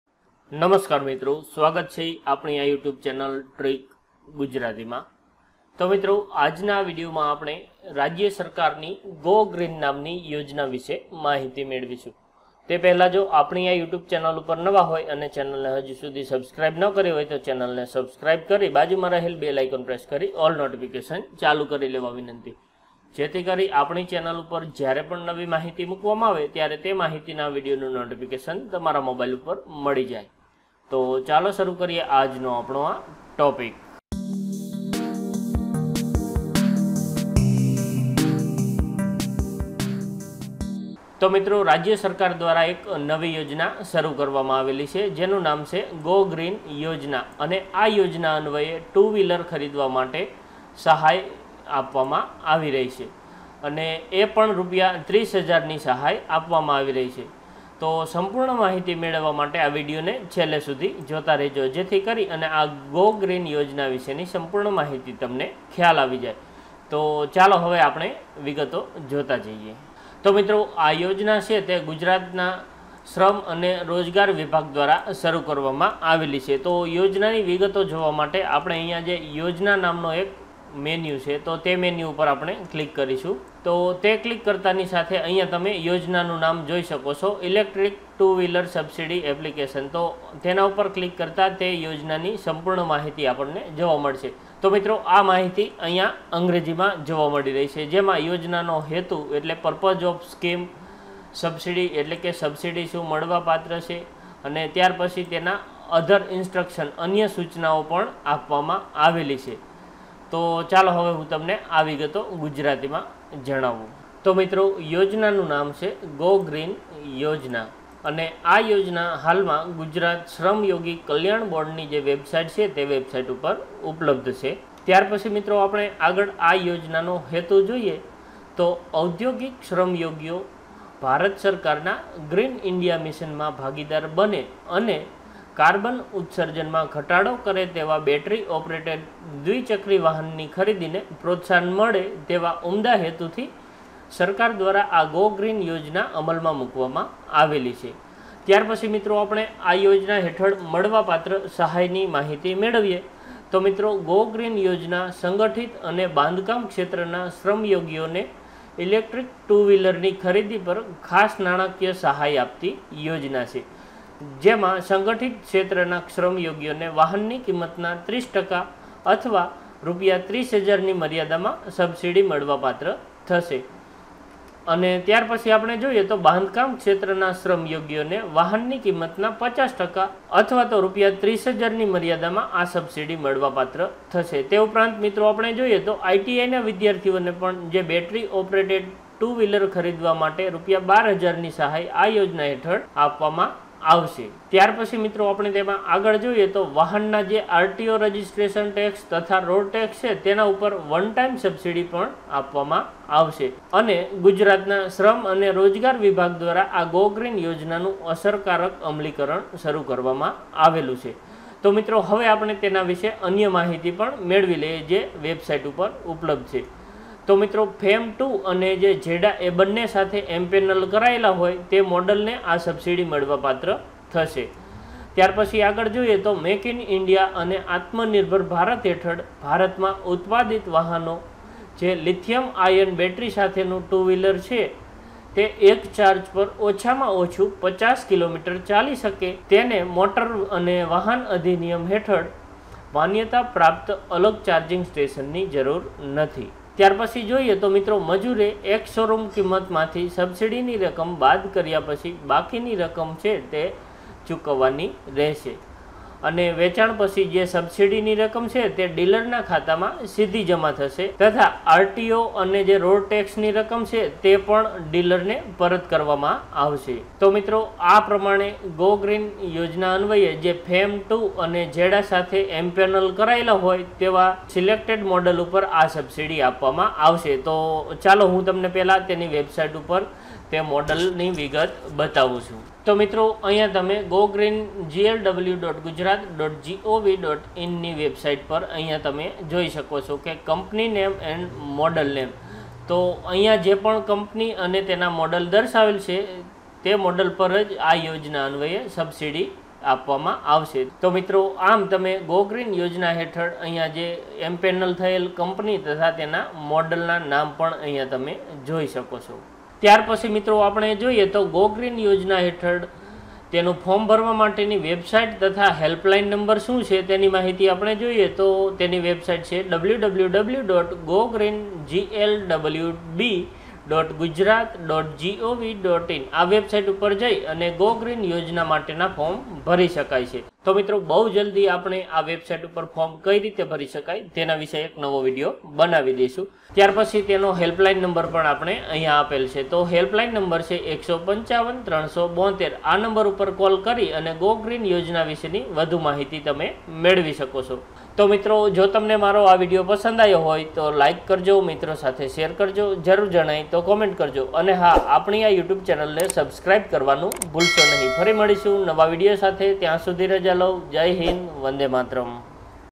Go Green Yojana Gujarat|30000 Subsidy On Two Wheeler Purchase|ટુ-વ્હીલર ખરીદવા મેળવો 30000ની સબસીડી
મિત્રો આ વિડયોમાં રાજ્ય સરકારની gogreen યોજના વિશેની માહિતી આપવામાં આવેલ છે